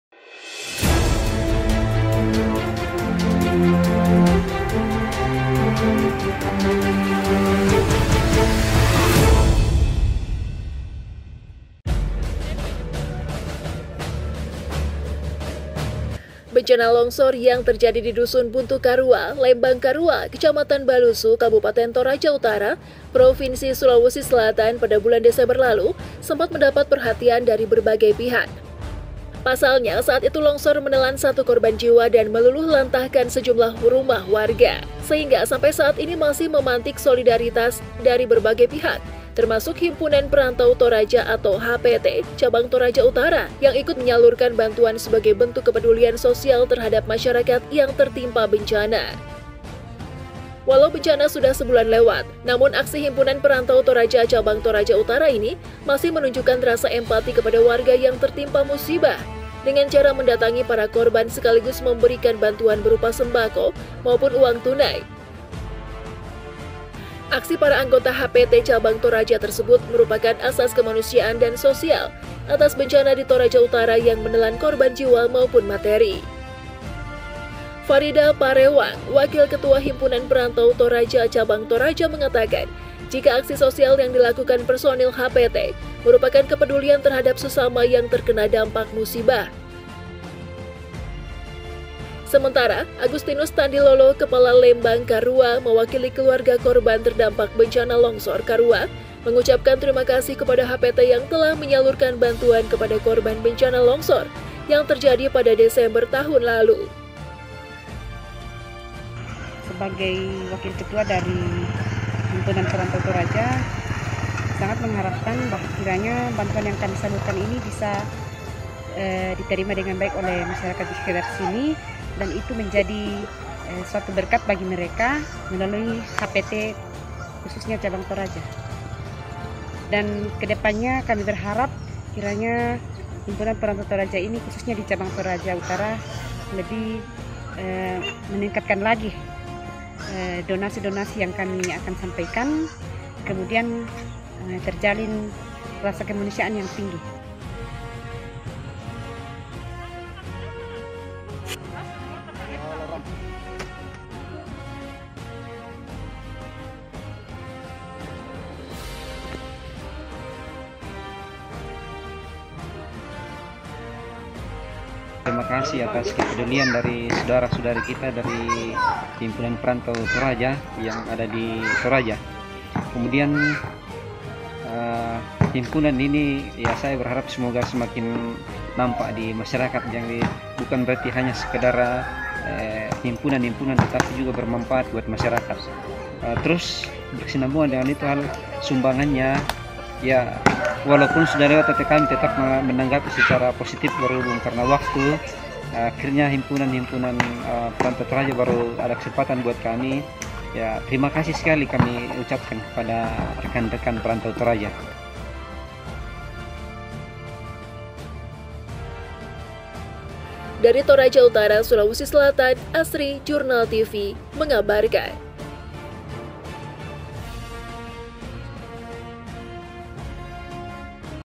PEDULI SESAMA, HPT CABANG TORAJA SALURKAN BANTUAN KE WARGA PASCA BENCANA LONGSOR DI KARUA
JURNALTIVI.COM merupakan Portal Berita, yang hadir dengan konsep multiplatform.